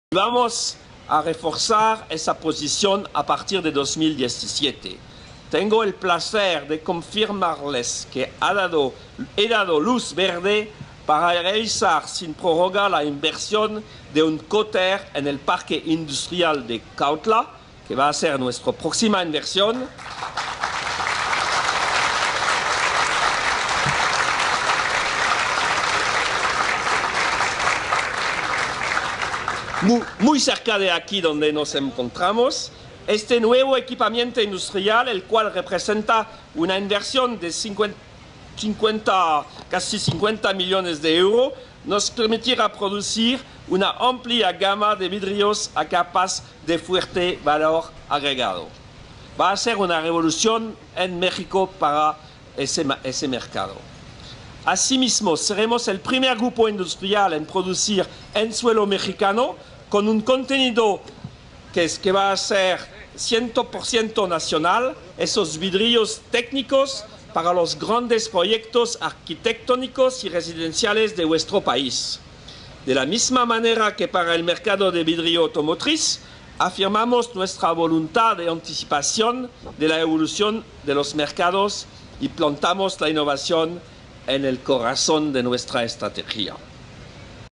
Saint-Gobain invertirá de 50 millones de euros en su planta en el Parque Industrial de Cuautla
En su 20 aniversario Saint-Gobain anuncia la inversión de 50 millones de euros en su planta en Parque Industrial de Cuautla.